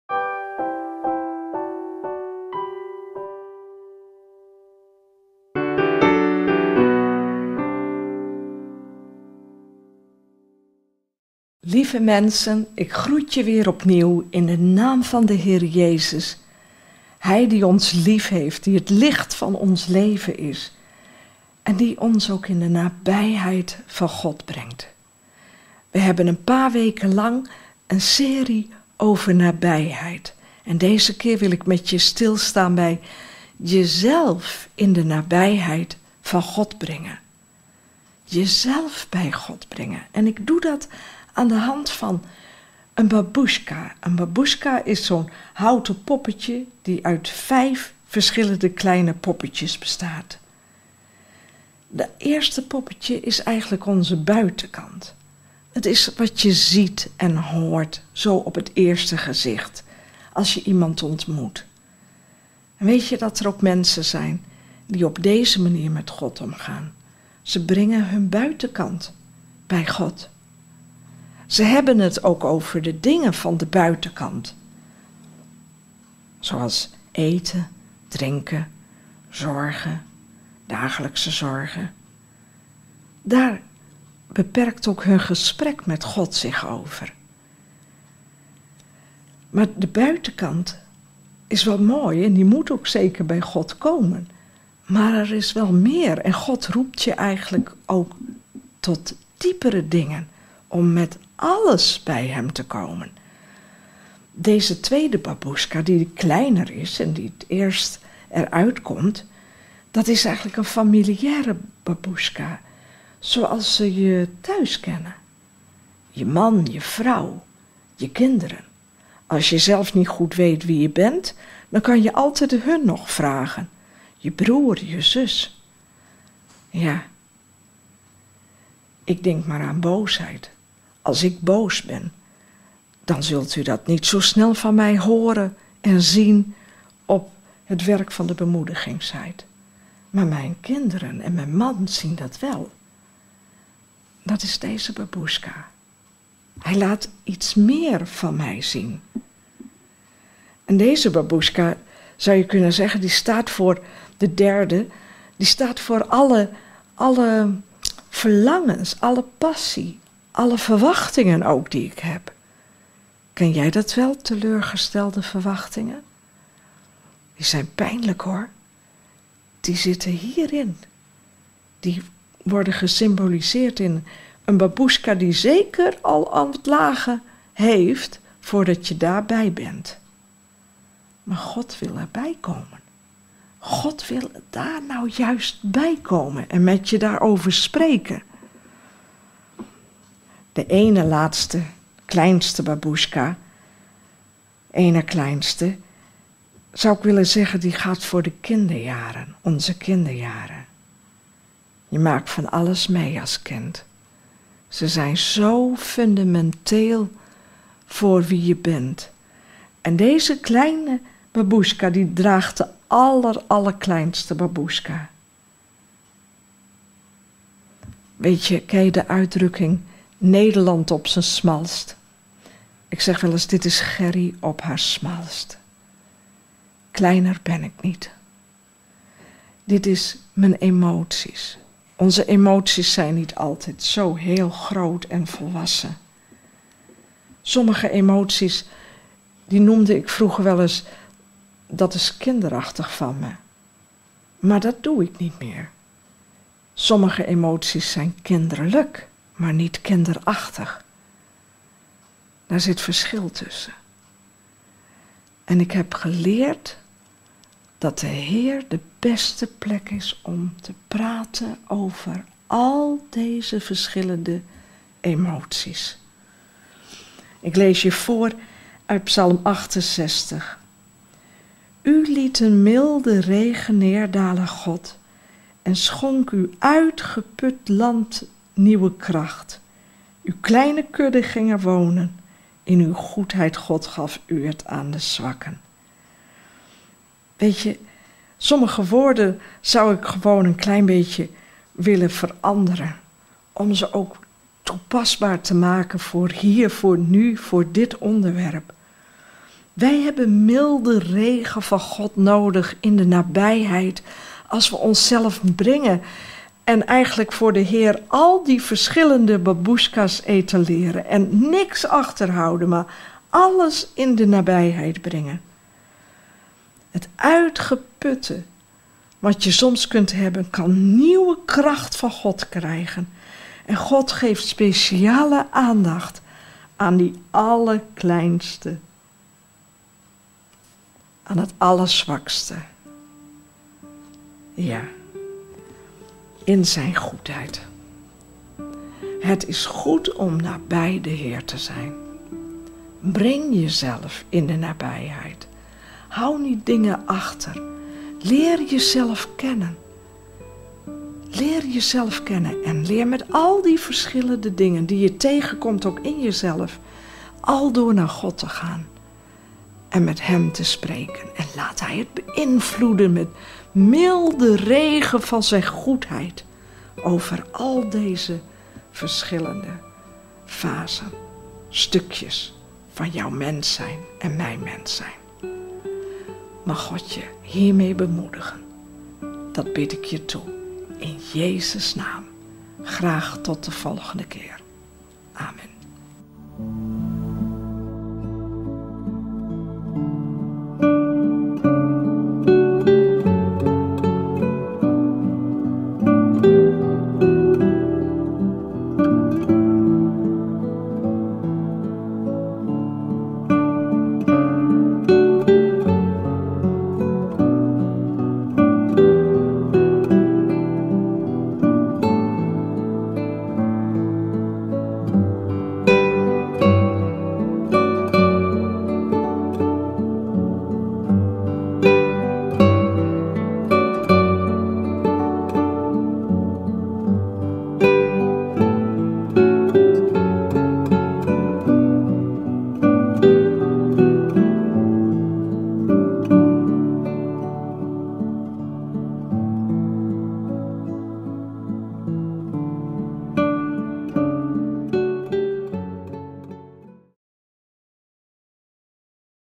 W38-12 Nabij jezelf
Jezelf in de nabijheid van God brengen, je buitenkant, maar ook de lagen onder die buitenkant... Deel 4 van de serie over nabijheid